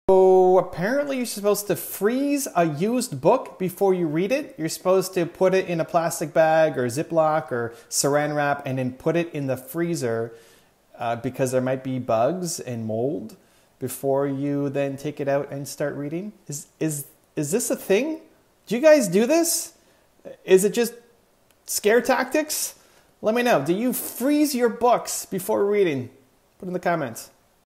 Do you freeze used books before you read them?
✎ In this video, we explore an unusual tip for book lovers! Have you ever heard that you should freeze a used book before reading it? Some people suggest putting the book in a plastic bag, like a Ziploc or wrapping it in Saran Wrap, and then placing it in the freezer. This is supposed to kill any bugs or mold that might be hiding in the pages. But is this really necessary, or just a scare tactic?

We'll dive into the reasoning behind this method and discuss whether it's a common practice among avid readers. You'll hear from book enthusiasts who share their experiences and opinions on this topic. Have you ever tried freezing your books, or do you think it's over the top?

Do you freeze your books before reading them? Is it something you'll start doing after watching this video? Let's get to the bottom of this quirky book care tip together!

★ BOOKS ★
• 254 Confidence

Much love,
Evan